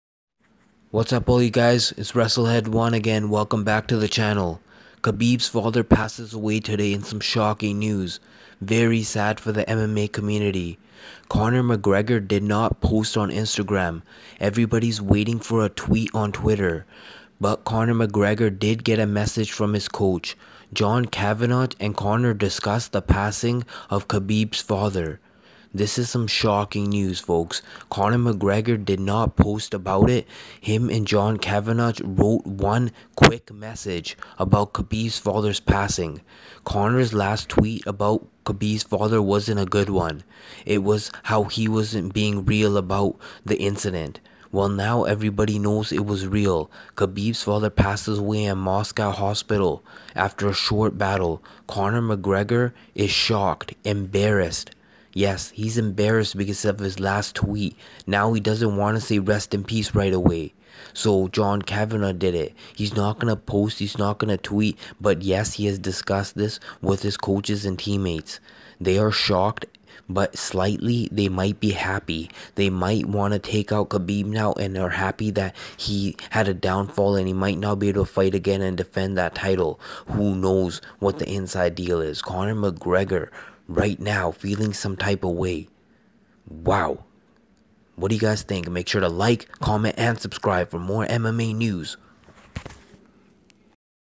Conor McGregor Reacts To Abdulmanap Nurmagomedov's Passing
He is embarrassed because of his last tweet.